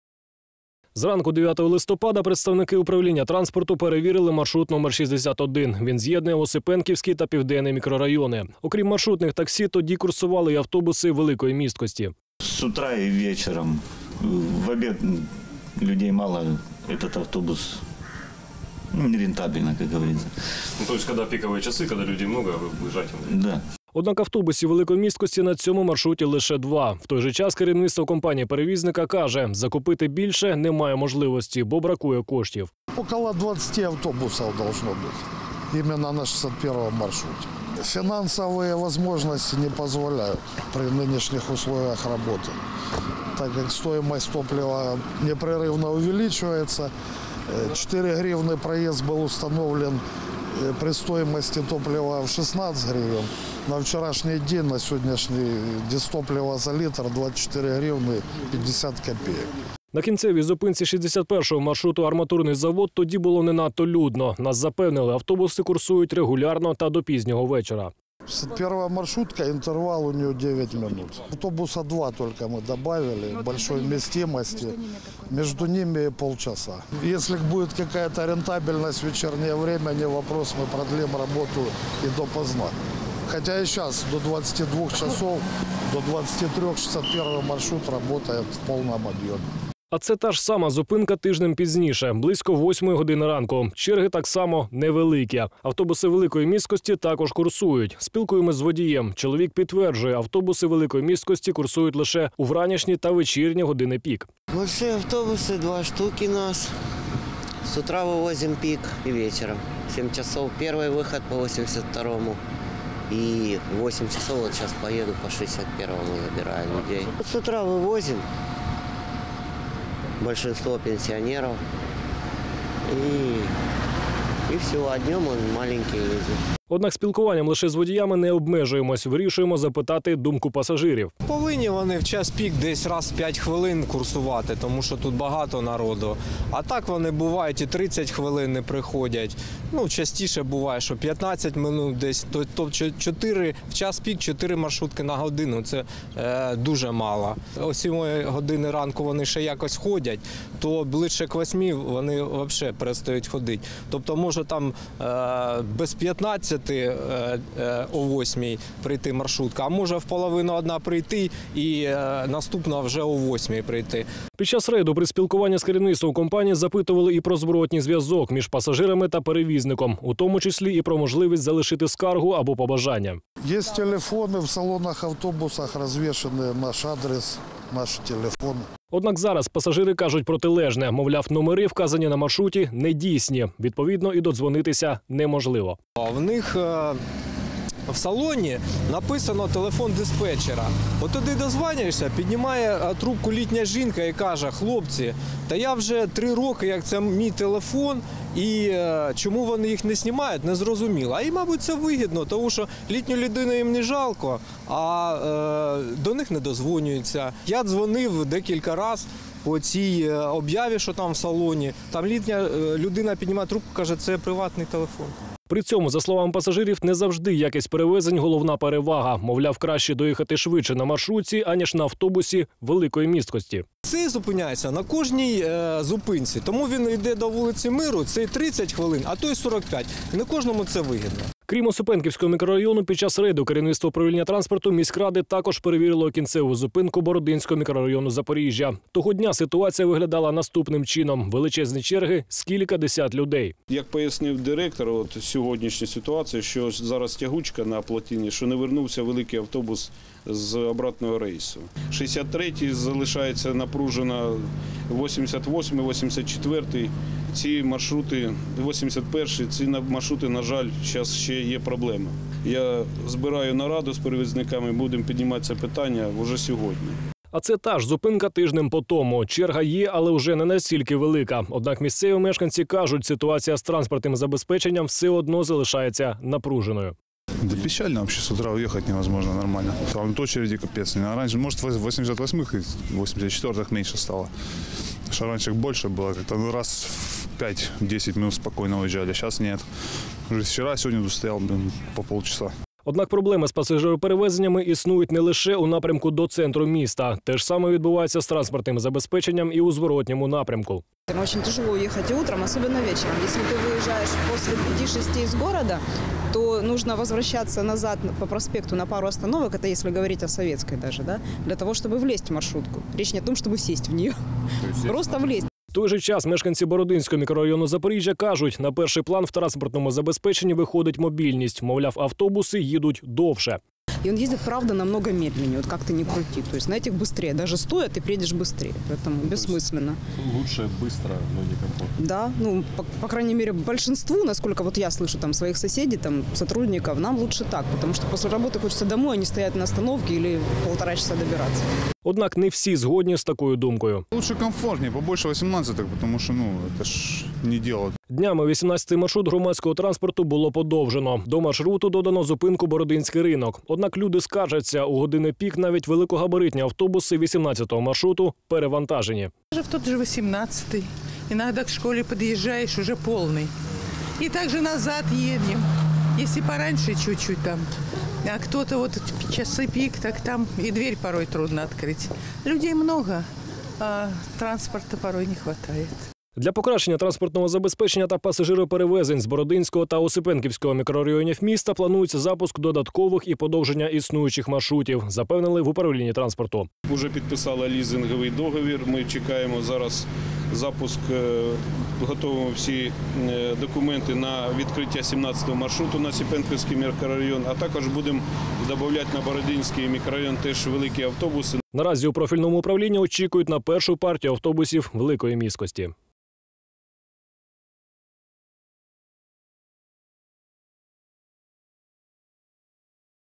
Транспорт у райони
Минулого тижня управління транспорту Запорізької міської ради влаштовувало рейди щодо перевірки роботи перевізників. Обрали декілька маршрутів, що обслуговують Осипенківський та Бородинський мікрорайони Запоріжжя. Однак керівники компаній-перевізників заздалегідь знали про майбутній рейд. І ба, тут тобі й автобуси великої місткості, і привітні водії, і обіцянки про злагоджену роботу маршрутів. Соціальний патруль влаштував власний, незалежний рейд тими ж маршрутами. Та перевірив, як компанії перевізники дотримуються даних раніше обіцянок. Чи курсує великогабаритний транспорт та чи дотримуються водії встановленого графіку?